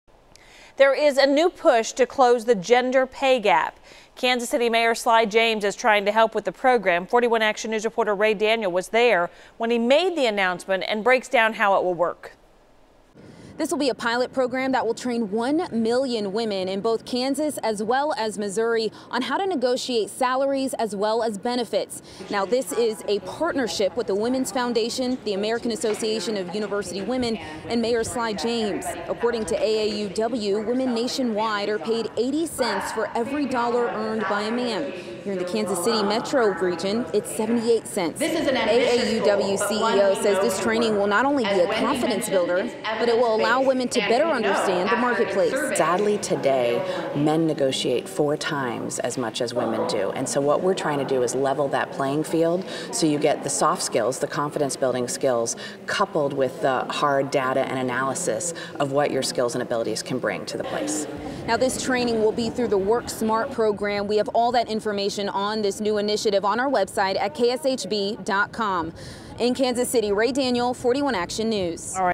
KC program takes aim at gender pay gap
KC Mayor Sly James is throwing himself behind a program aimed at closing the gender pay gap.